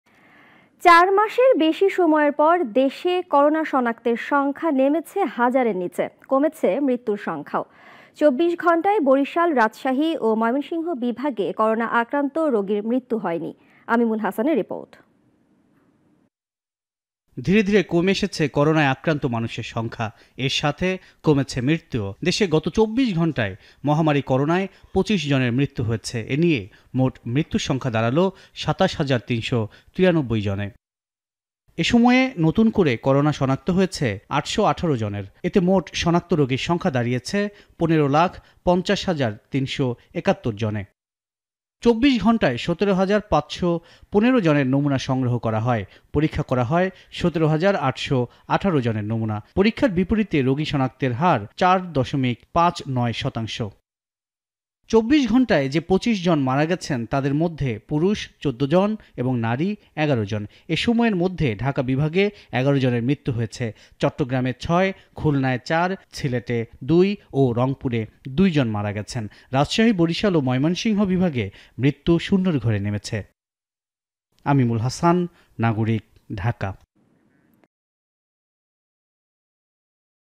করোনায় দৈনিক শনাক্ত হাজারের নিচে নামল, মৃত্যু ২৫ | Corona Update | Covid19 | Nagorik News
চার মাসের বেশি সময়ের পর দেশে করোনা শনাক্তের সংখ্যা নেমেছে হাজারের নীচে। কমেছে মৃত্যুর সংখ্যাও। ২৪ ঘণ্টায় বরিশাল, রাজশাহী ও ময়মনসিংহ বিভাগে করোনা আক্রান্ত রোগীর মৃত্যু হয়নি।